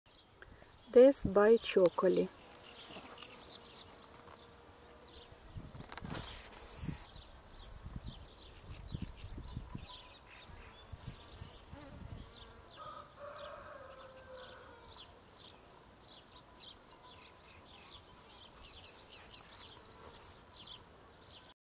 Death By Chocolate, Byanko’02. Дес Бай Чоколіт. Карликовий ірис.
Дес Бай Чоколіт
Death By Chocolate, Byanko’02.  S – шоколадні з фіолетовими розводами, F  - кофейно оксамитові теж з фіолетовими розводами і світлішими краями. Хвилястий. Гірчично-коричневі борідки.